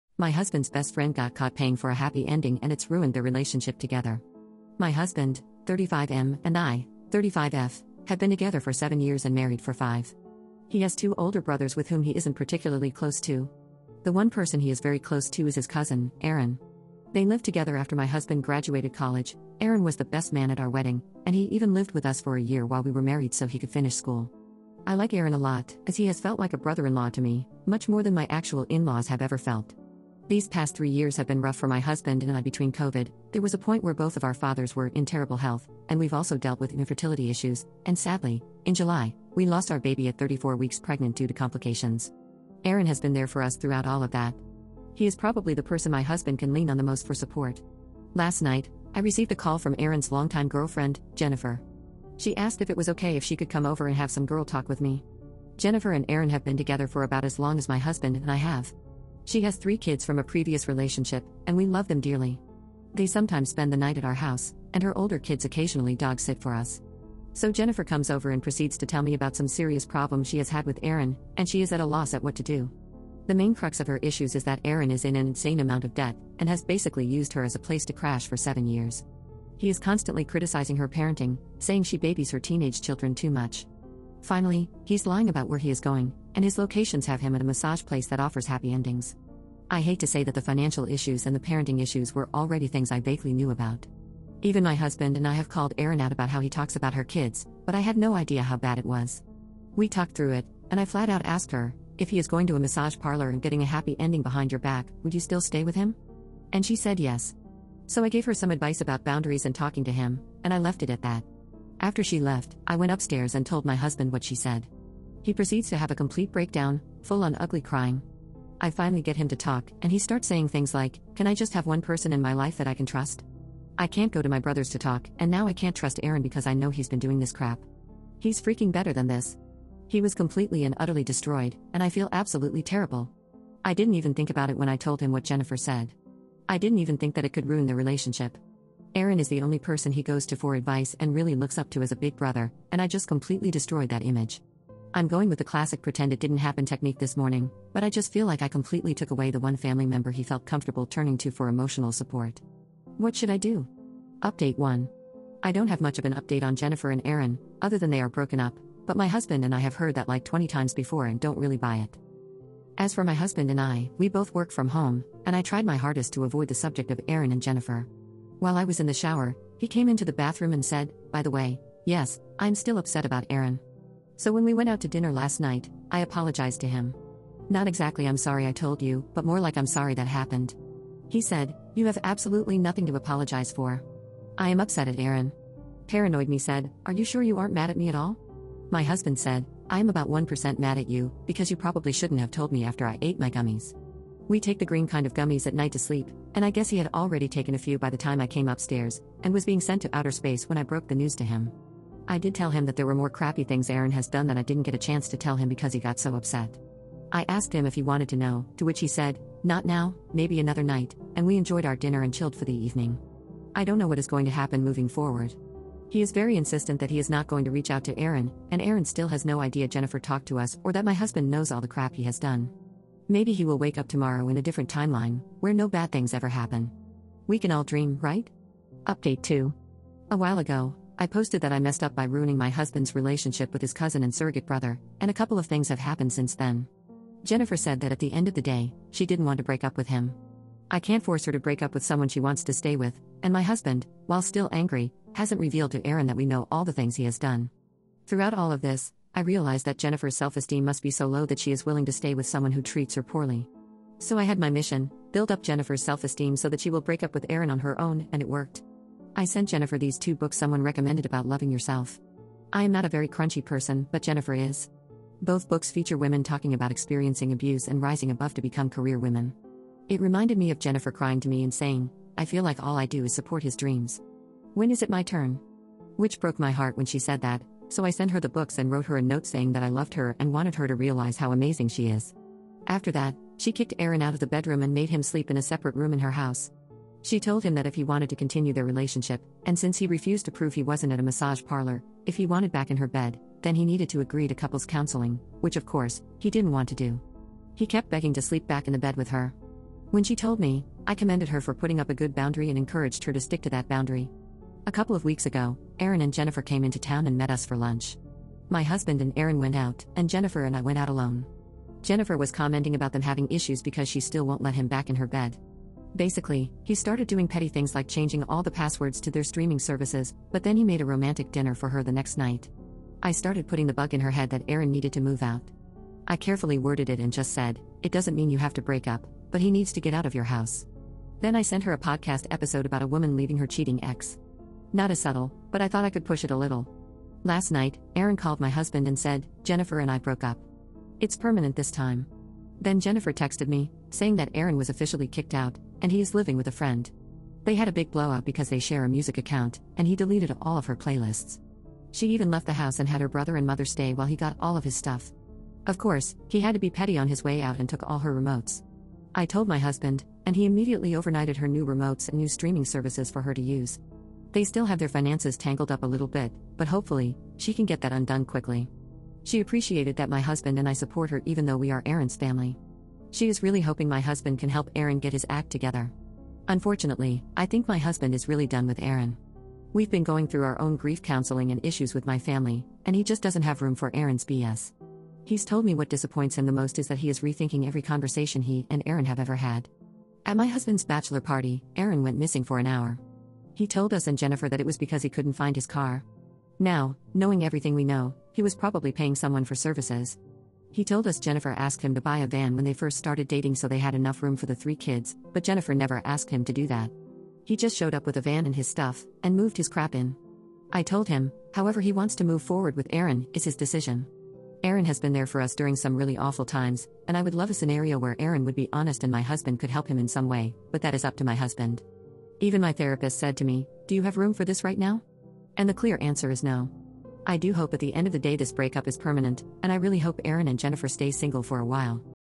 My husband's best friend got caught paying for a happy ending and it's ruined their relationship...
My husband's best friend got caught paying for a happy ending and it's ruined their relationship together. Plus two updates.

u/LunaNova5726.